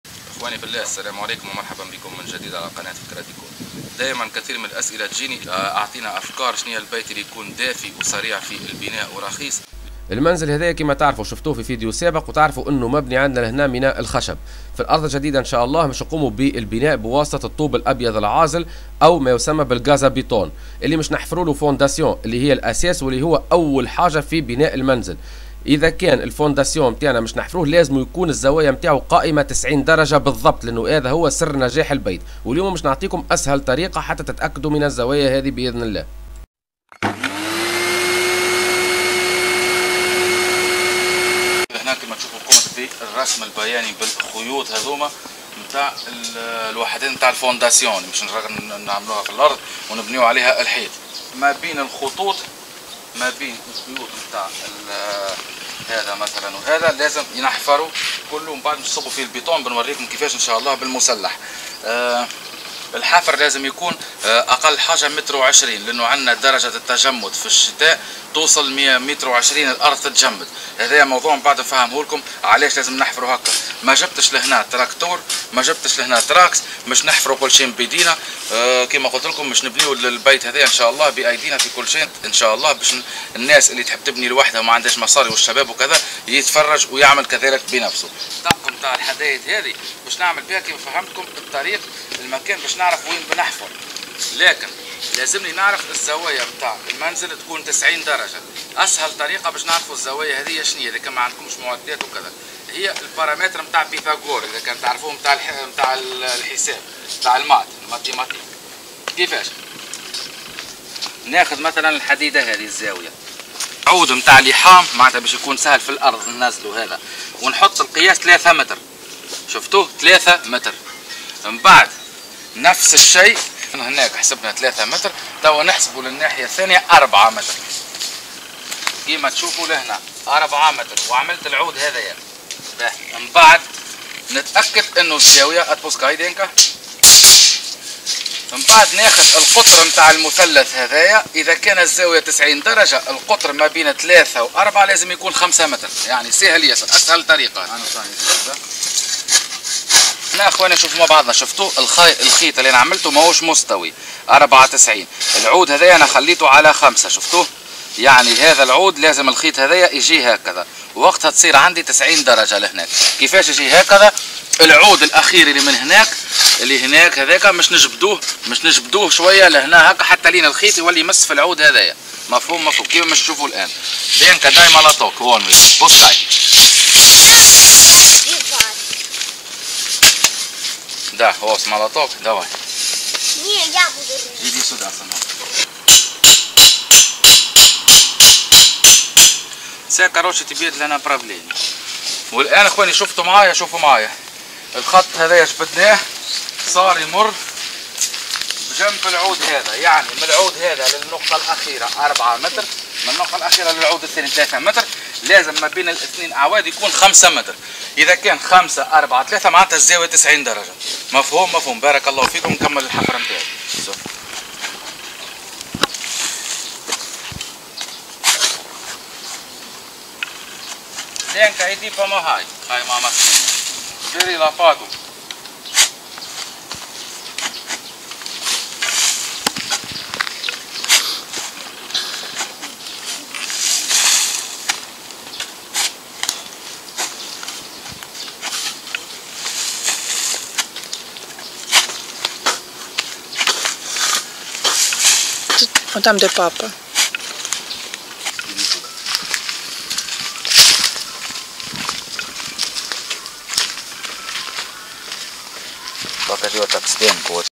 تحدي بناء منزل رائع و رخيص بنفسك2: ماذا تفعل قبل أن تحفر أساسات منزلك وتبدء في البناء ؟
أهم شي عليك التاكد منه قبل أن تبدا في تسليح خراسانة أساسات بيتك إذا أهملت تقاطع زوايا بناء البيت فسوف تخسر الكثير من الوقت والمال حتى تستطيع تعديلها في ما بعد أعمال بناء منزل جديد دافي ورخيص وبناء بيت جميل وعصري سوف ابني بيتي بنفسي أحسن من أي عامل بناء